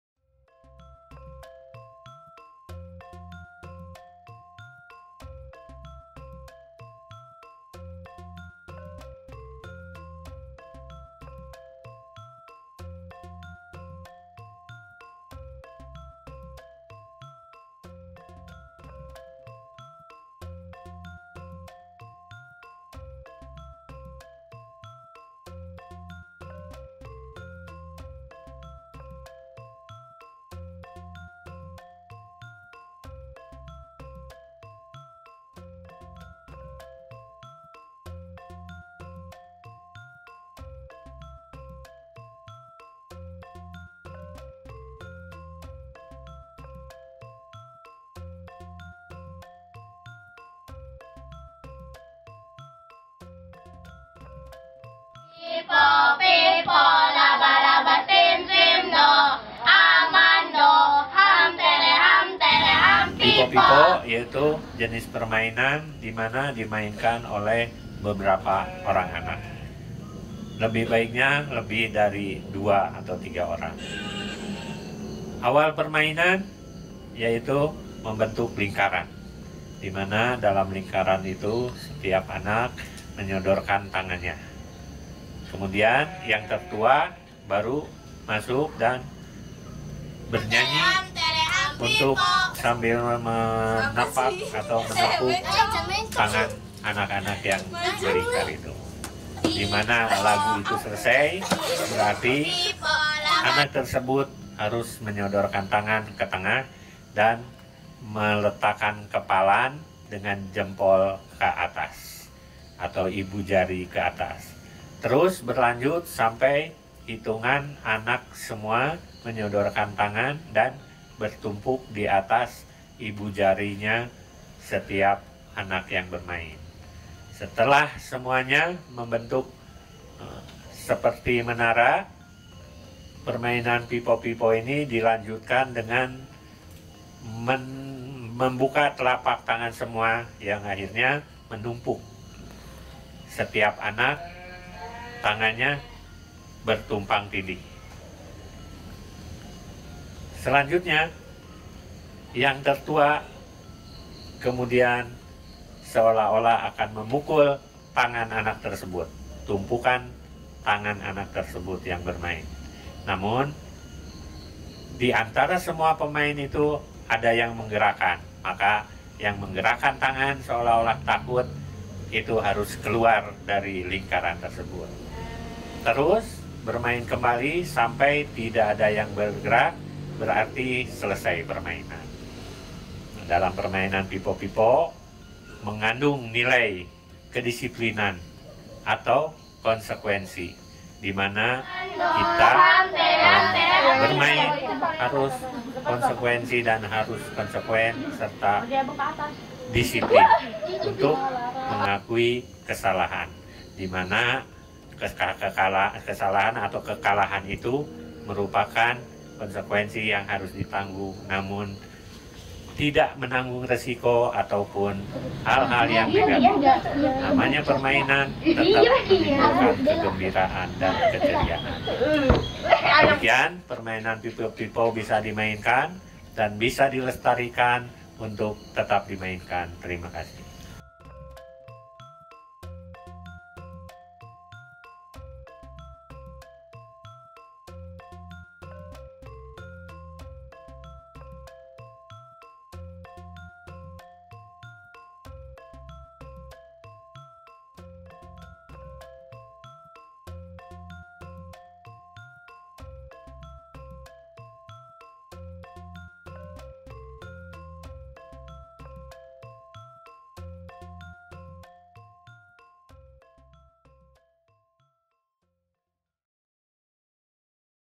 Permainan Tradisional Jawa Barat - Pipo Pipo
Pipo-pipo merupakan salah satu permainan tradisional yang ada di Jawa Barat.